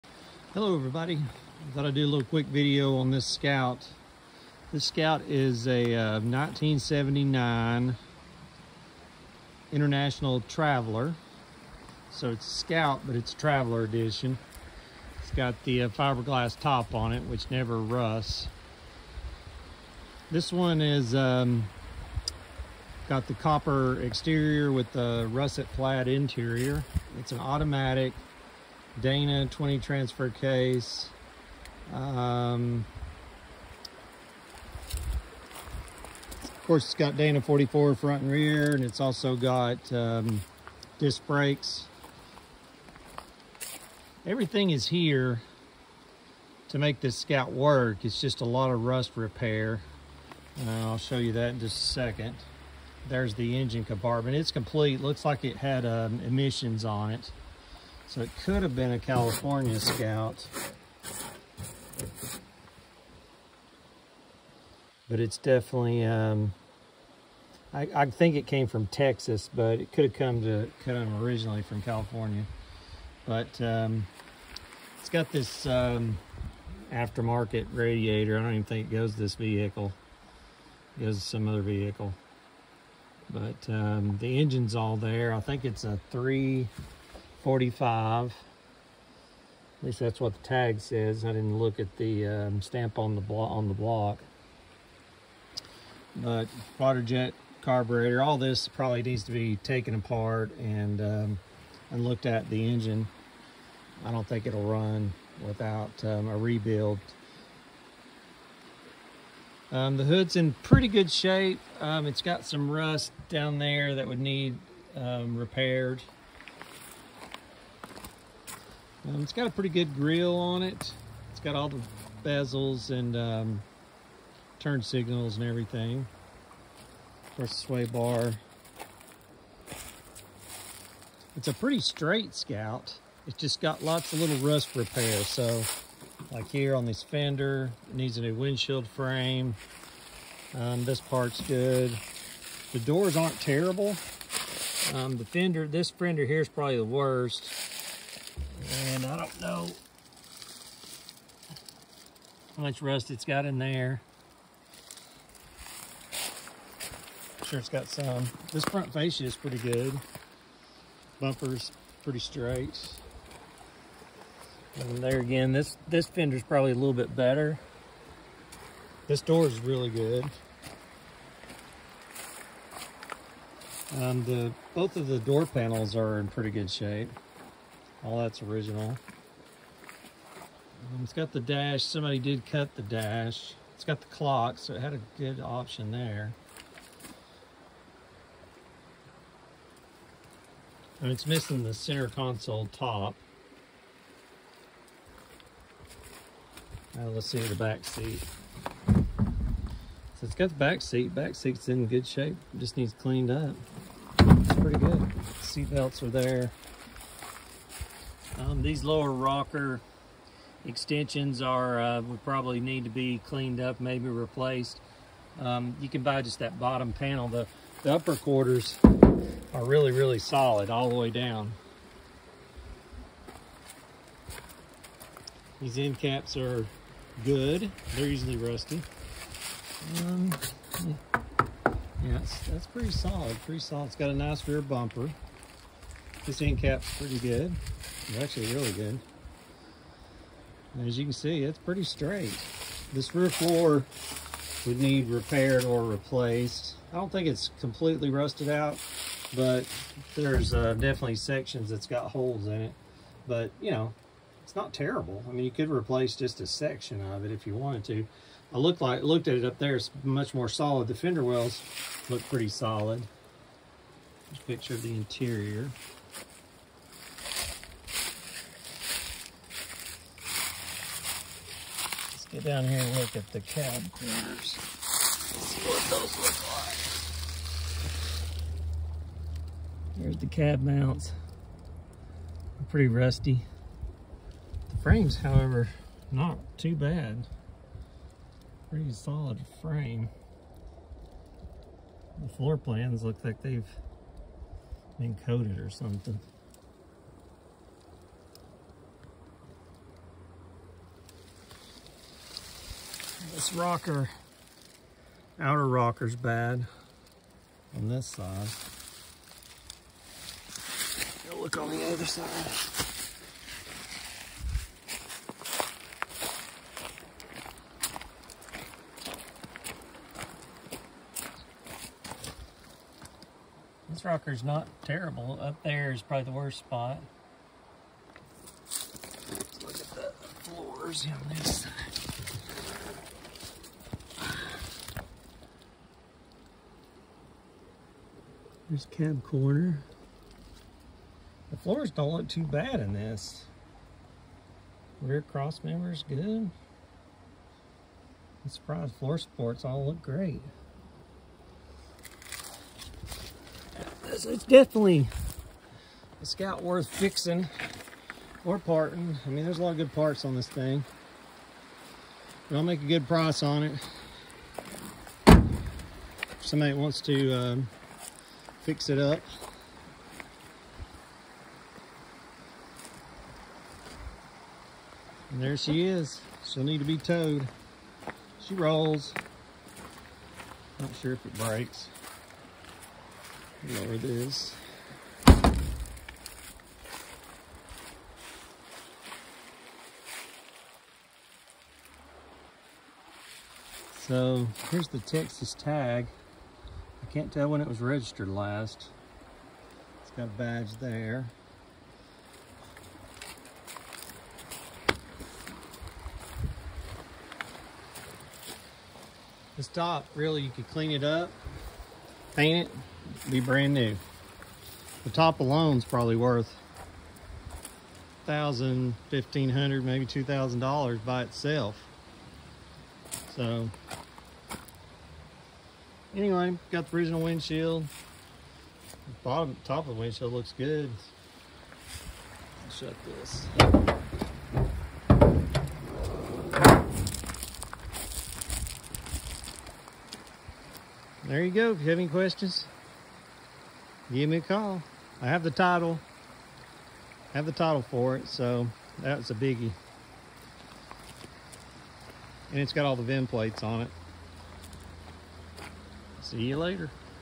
1979 Scout Traveler - Rusty but lots of good parts
Very Rusty but lots of good parts and noting is un-restorable with a work. $2500